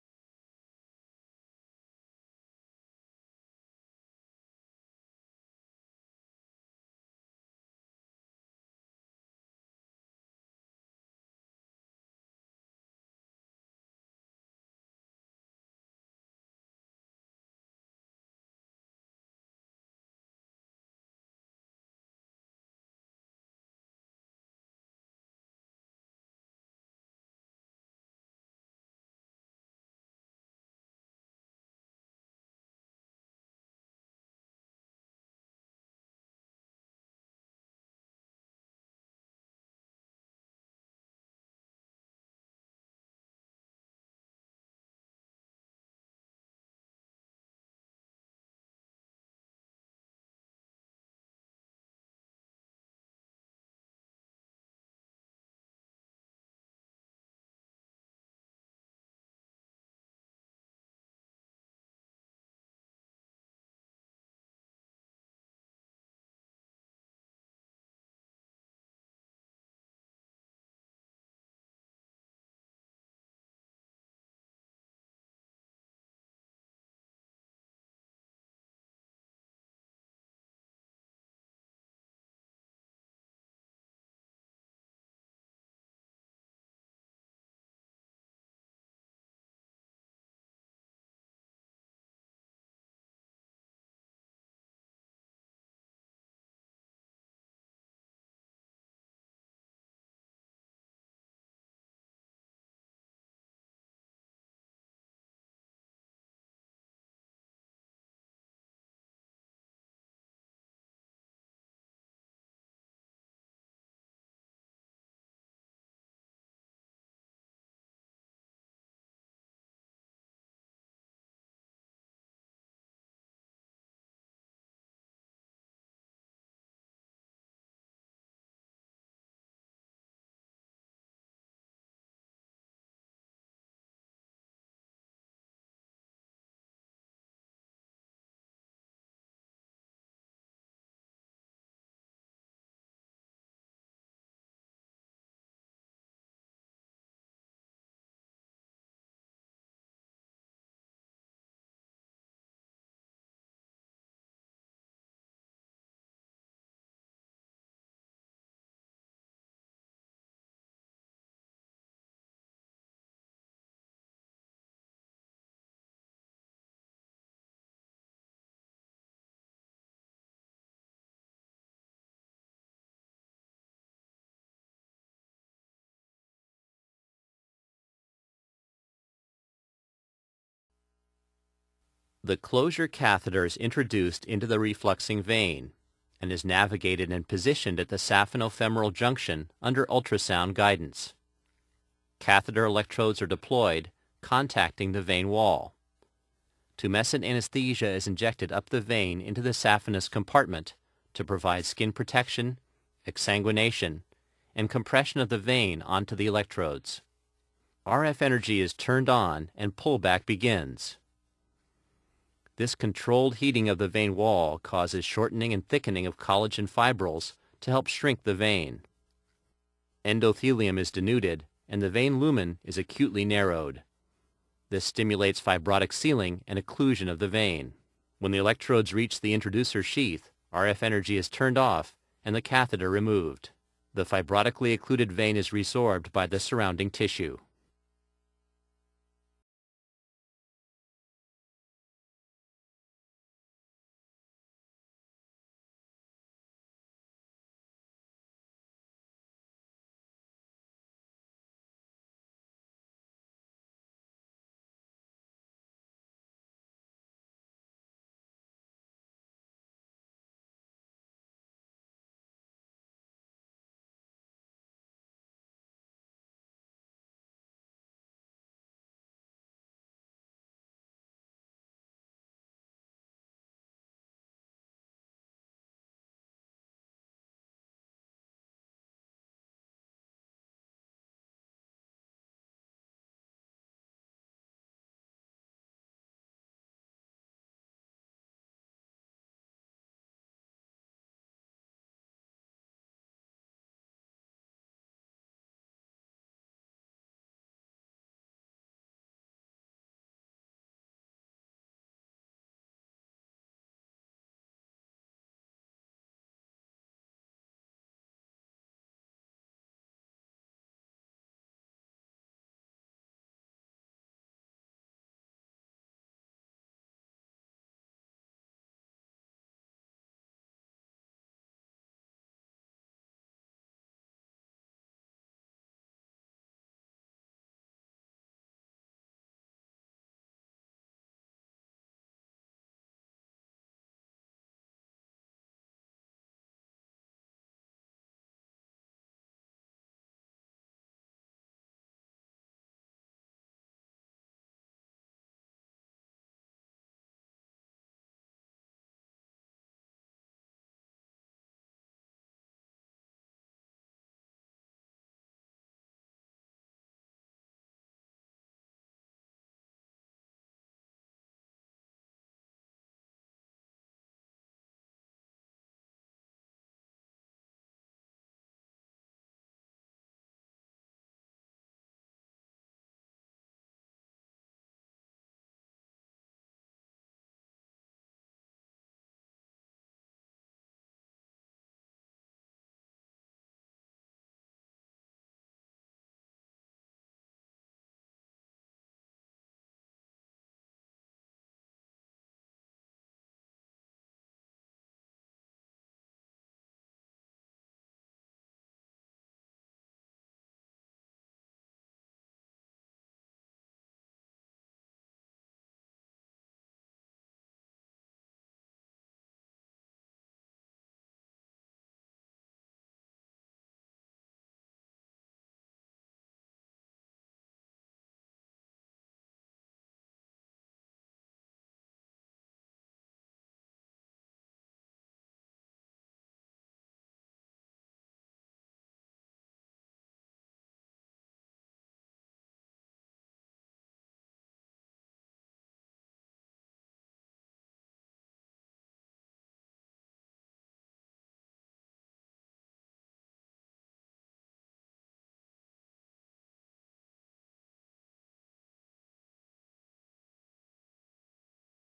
Surgical Options for Venous Disease
Slide presentation by Sandra Carr MD on the surgical options for venous disease.